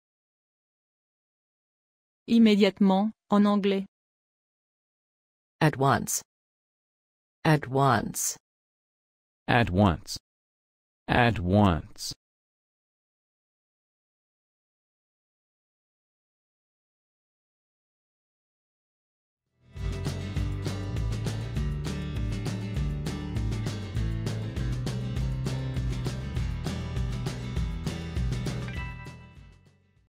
Comment dire immédiatement en anglais
Comment dire correctement immédiatement en anglais? Ecoute avec nous.
Quelle est la traduction correcte du mot immédiatement du français vers l'anglais? Parlez comme les Américains. Aussi, comment écrire correctement le mot at once?